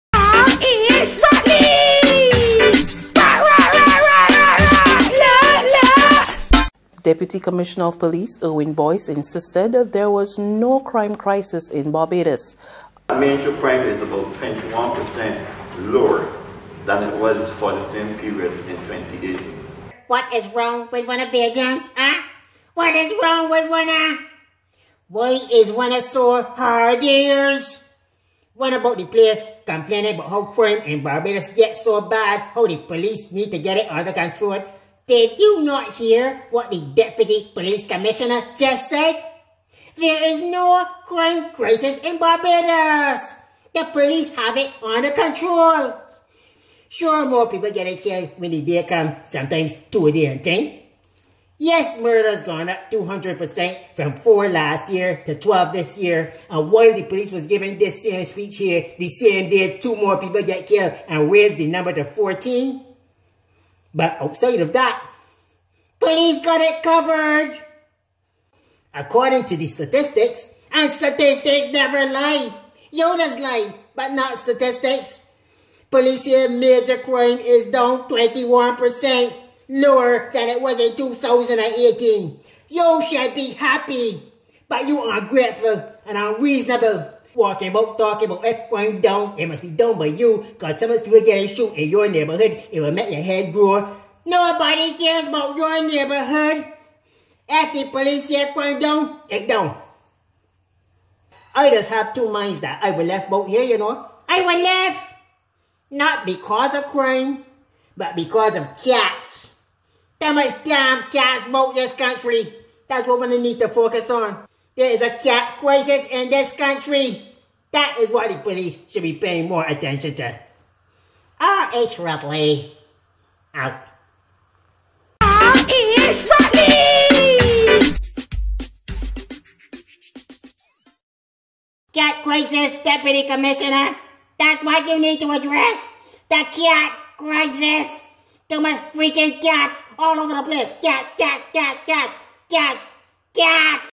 RH Ratley Rants on Barbados High Crime Rate
Only Bajan Rat RH Ratley can be funny but yet serious at the same time, as he tackles the high rate of crime  in Barbados and lampooning the Police response. Whether its a Crime or Cat Crisis, check this out